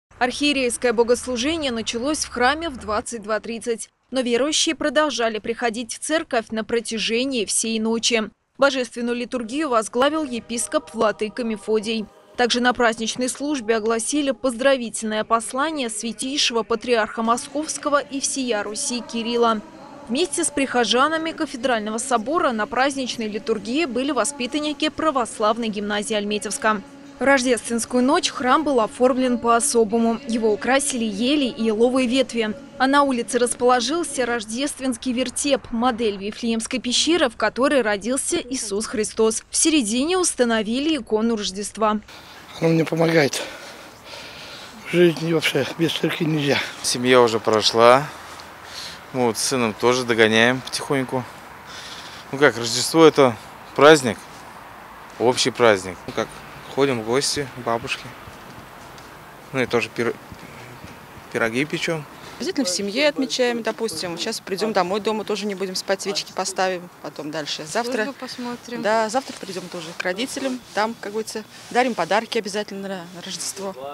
Светлый праздник: альметьевцы – о том, за что они любят Рождество
Православные христиане Альметьевска отметили Рождество. В праздничную ночь центральный храм города – Собор Казанской иконы Божией матери – посетили сотни человек.